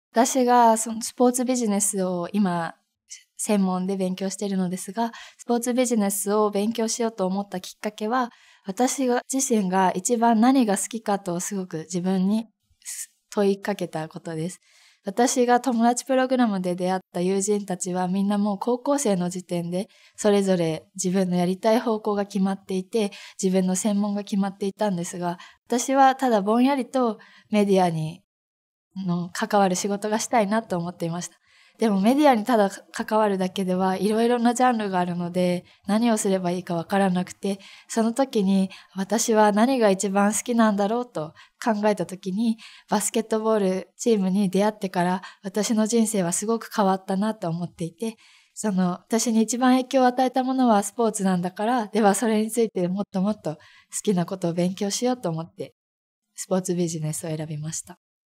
Why Sports, Media and Entertainment (Japanese) | Sports Marketer Chisa Funada | Career Girls
Watch Chisa Funada, a student at Keio University in Tokyo, majoring in Sports Business. After getting to know the staff of a pro basketball team in her hometown and learning about the field’s opportunities, she became interested in the sports business.